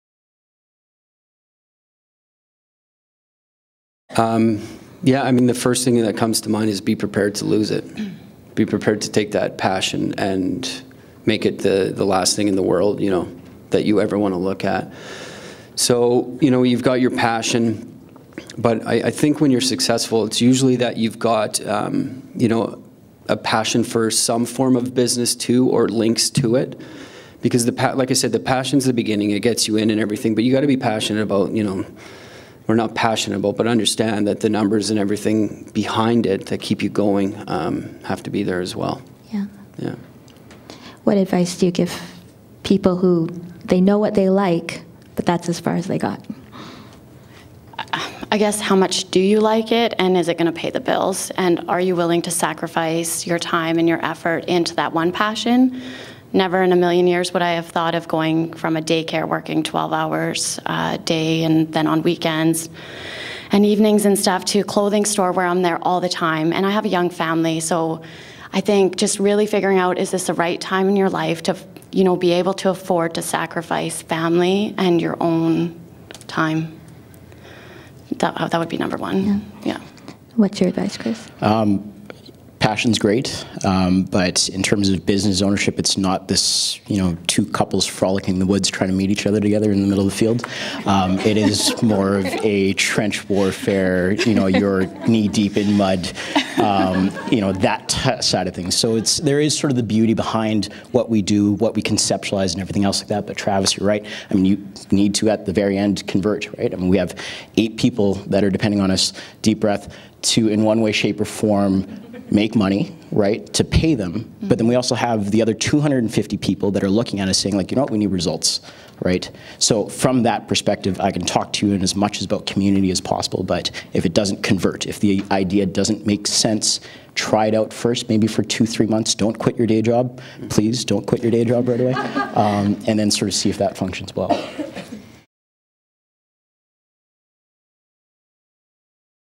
What advice do you have for someone looking to turn their passion into a business?
CapitalIdeasEdmonton.com — Chris Tse, Kristi Wozniak and Travis Blake share advice for turning your passion into a business.

Christ Tse of Blitz Conditioning, Kristi Wozniak of KEYLIME Athletic Wear and Travis Blake of R3 Deconstruction & Demolition were on a panel entitled: "How do I turn my passion into a business?" It was held at the Edmonton Journal on April 17, 2013 and was moderated by Karen Unland.